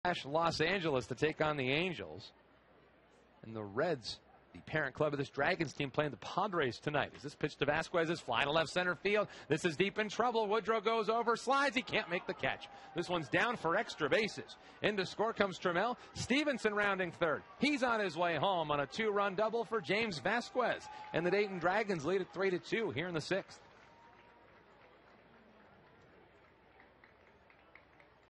Vasquez brings in two runs with double for Dayton
6/12/17: Dayton's James Vasquez rips a double to left field to bring in two runs in the sixth inning against West Michigan